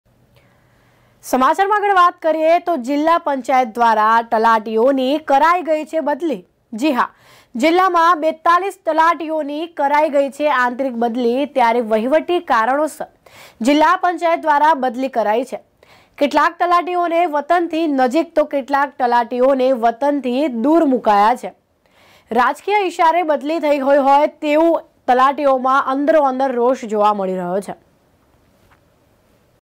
સાબરકાંઠા:જિલ્લા પંચાયત દ્વારા તલાટીઓની કરાઈ બદલી
જિલ્લામાં 42 તલાટીઓની કરાઈ આંતરિક બદલી
વહીવટી કારણોસર જિલ્લા પંચાયત દ્વારા બદલી કરાઈ
કેટલાક તલાટીઓને વતનથી નજીક તો કેટલાક તલાટીઓને વતનથી દૂર મુકાયા

Top Headlines
News Flash
Also check out ✅ our official Divya Bharat News site: